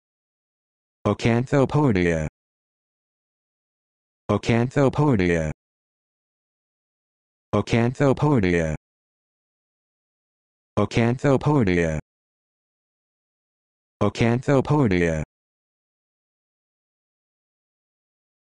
How to Pronounce acanthopodia
This video shows you how to pronounce acanthopodia